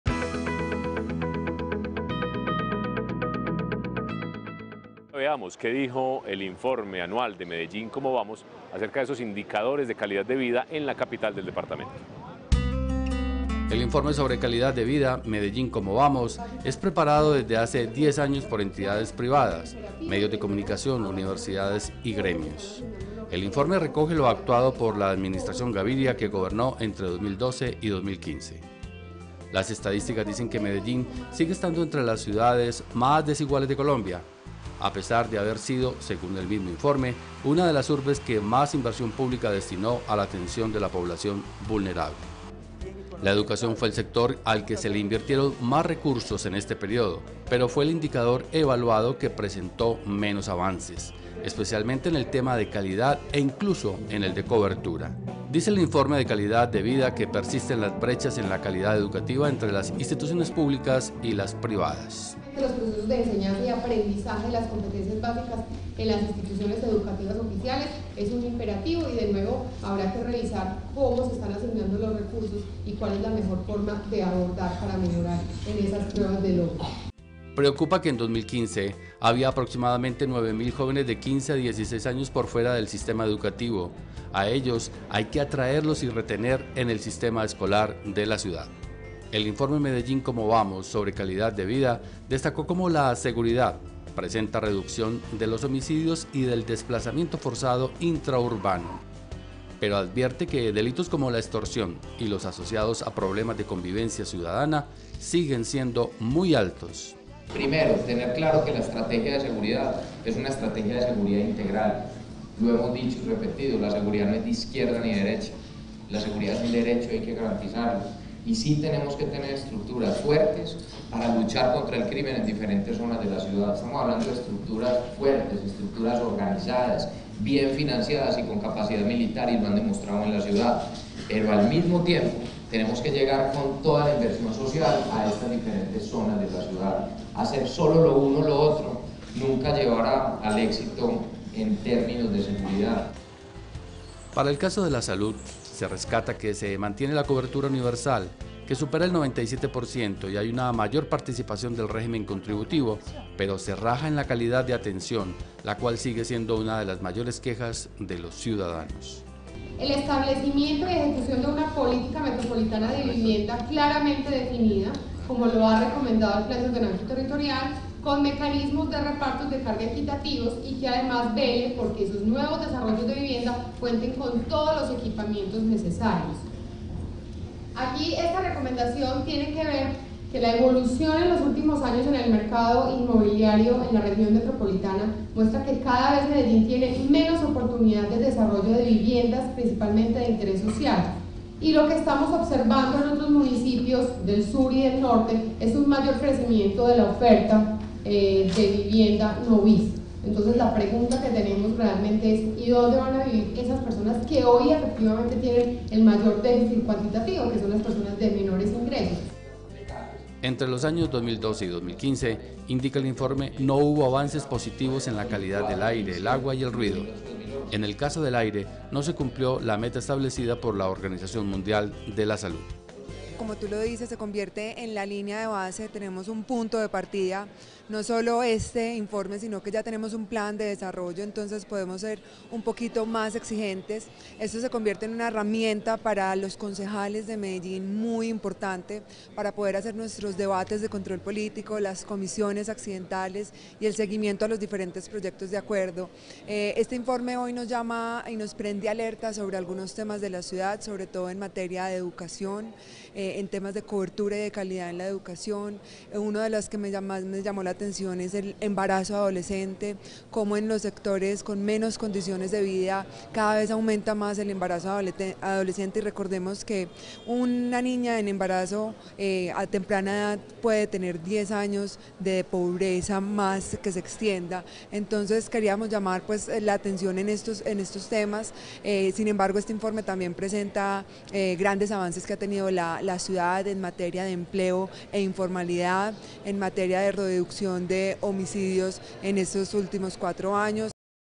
De Acuerdo: Retos a partir de informe Medellín cómo vamos
La presentación del informe Medellín cómo vamos plantea una serie de retos para el próximo cuatrienio. Seguridad, movilidad y ejecución son algunos de los puntos que la ciudadanía evaluó de años anteriores y que ahora deben ser tenidos en cuenta.